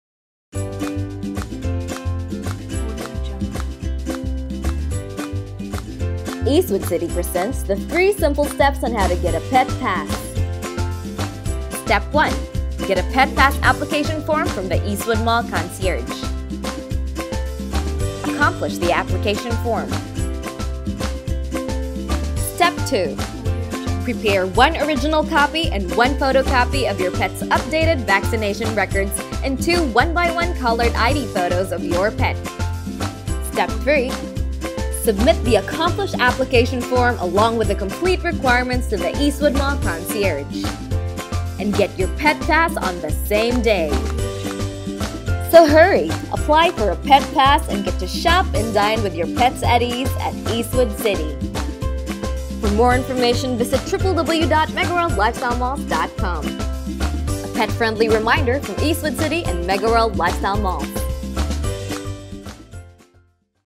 3 Simple Steps on How to Get a Pet Pass
THE NEW AND UPGRADED MEGAWORLD LIFESTYLE MALLS PET PASS IS NOW AVAILABLE AT THE CONCIERGE!

It’s free and it comes in a convenient card format, plus its validity is extended until the expiry of your pet’s rabies vaccination.

1. To get the NEW Pet Pass, please present the following requirements:
   a. Original updated vaccination record of your pet (original copy only; photo and scanned or soft copy is not allowed)
   b. One (1) photocopy of your pet’s updated vaccination record
      • Front page
      • Innermost portion clearly showing your pet’s latest rabies vaccination
   c. Two (2) 1x1 colored photos of your pet

2. Accomplish and sign the Pet Pass Application form at the concierge.

3. Submit the requirements and accomplished Pet Pass application form to the concierge.

You may also download the Application form, then scan and email the accomplished form to the concierge where you will claim your card ( , or )